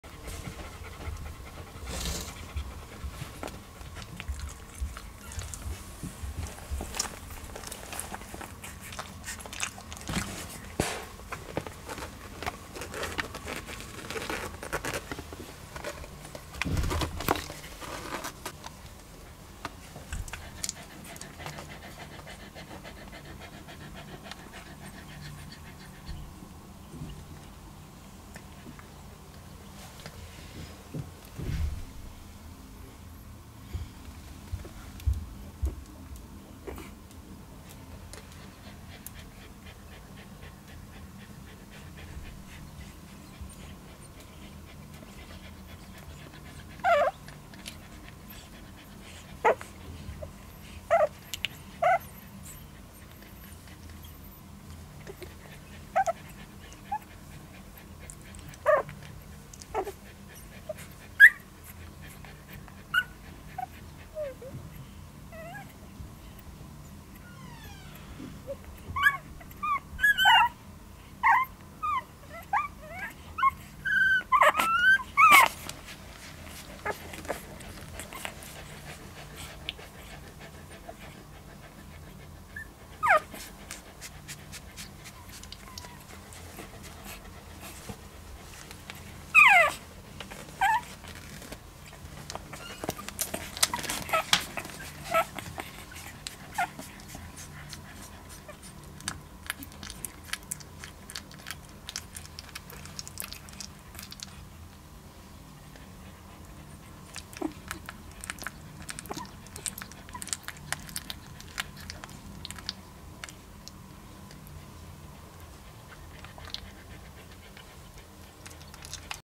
Hillan Pennut 2.9.2020 2-viikon ikäisiä
Nyt on pennut punnittu ja ensimmäinen madotus suoritettu 😝silmät on avautunut melkein jokaiselta , hienoa😃
Pentujen paino 660-700g isoja pötköjä jo.